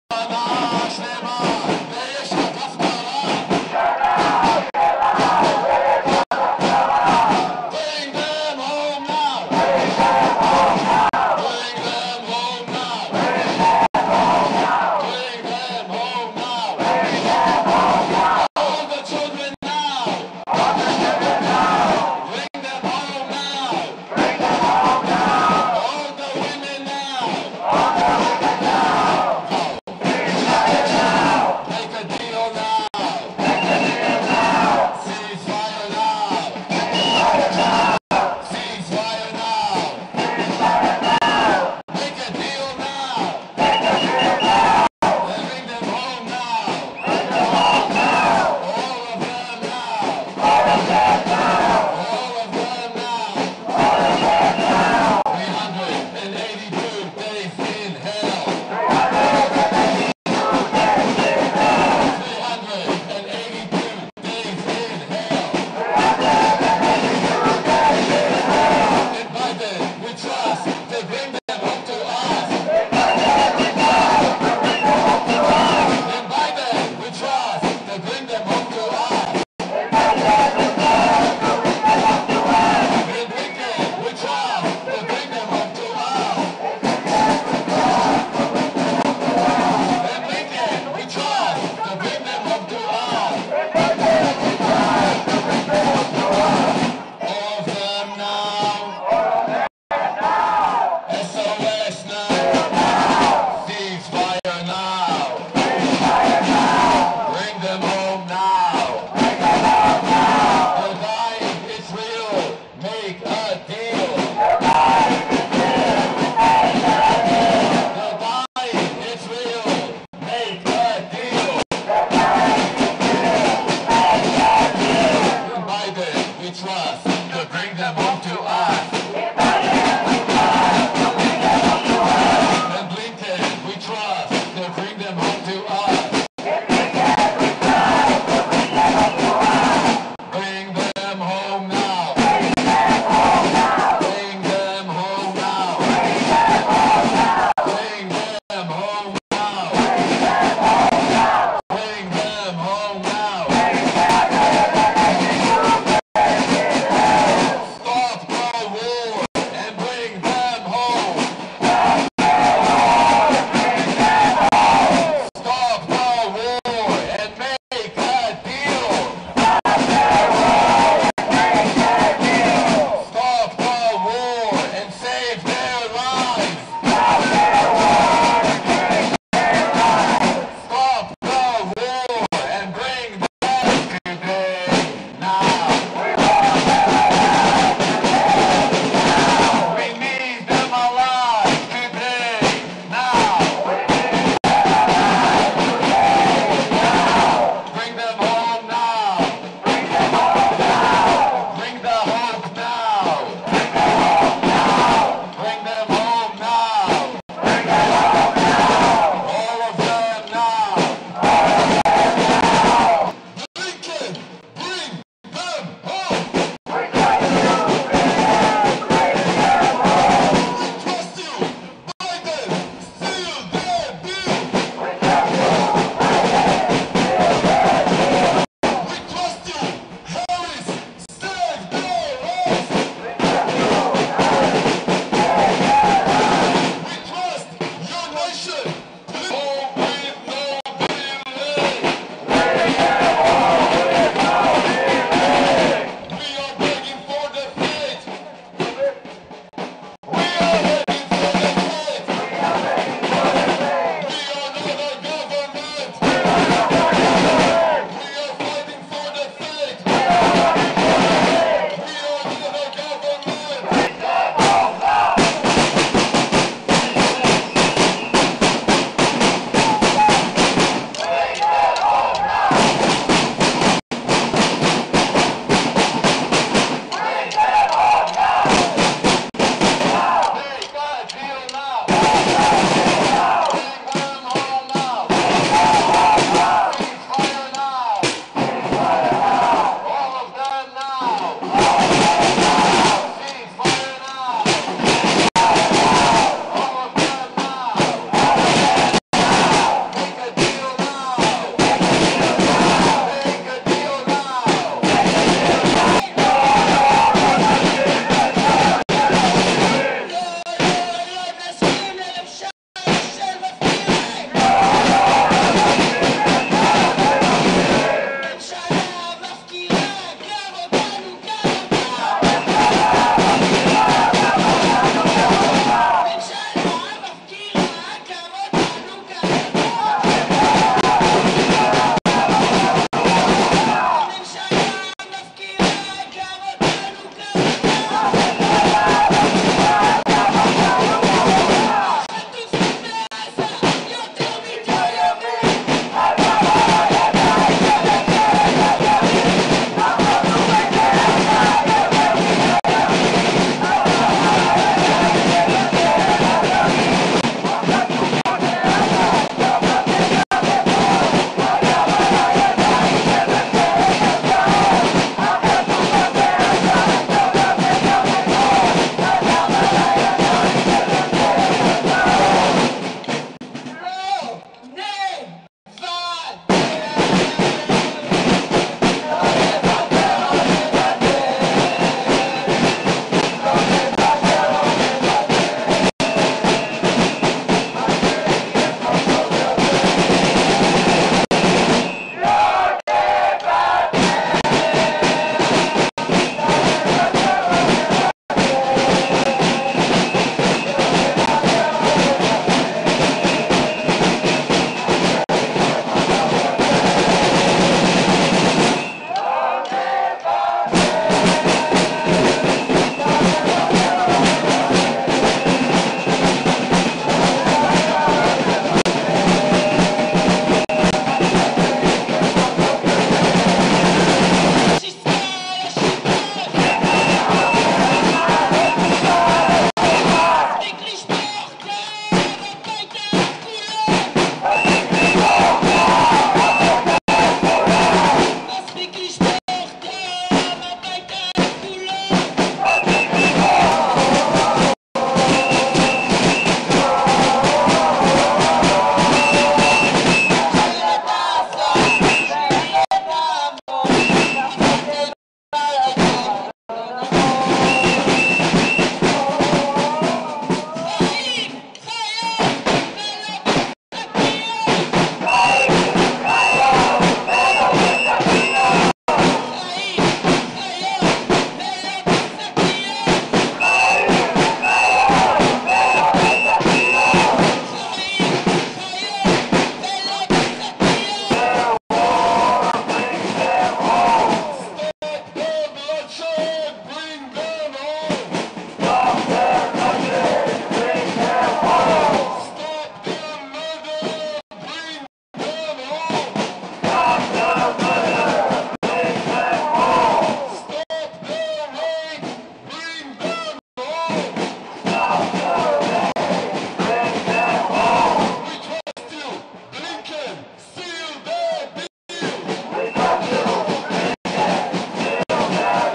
Massive protests against Netanyahu | Outside Tel Aviv Blinken's hotel
Israeli hostages’ families hold a protest outside Tel Aviv hotel
U.S. Secretary of State Antony Blinken's is staying there